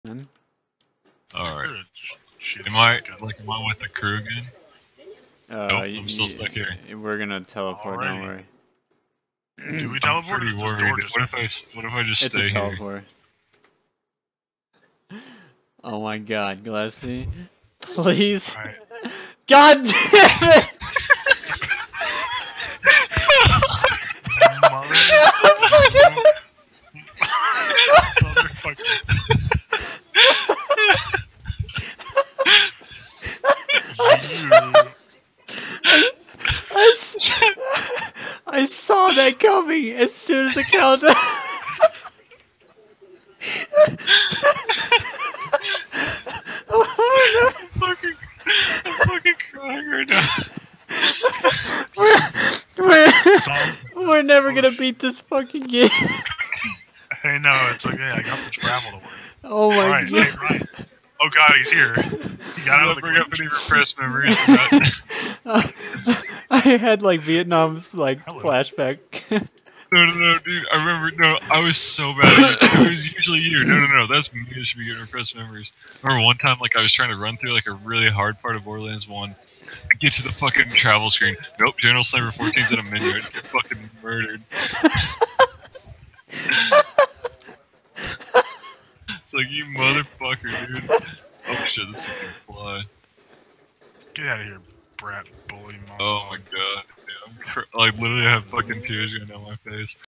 Gillespie Pauses The Game
Gillespie brings back memories of never being able to get past a countdown in a multiplayer game.